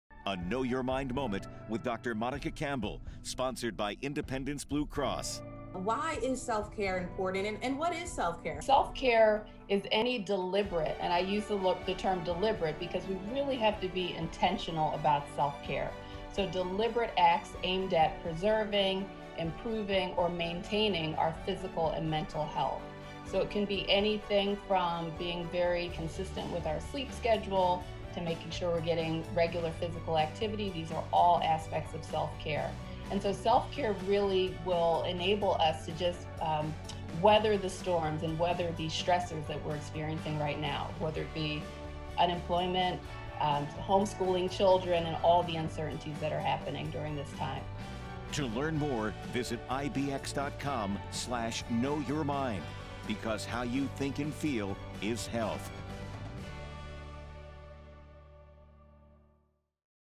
Dr. Monica Campbell on intentional self-care
Self-care can take many forms, like healthy eating, exercise, and meditation. In this KnowYourMind moment, Dr. Monica Campbell reminds us why it’s important to be intentional about preserving our physical and mental health through acts of deliberate self-care.

For our blog with health and wellness resources and insurance tips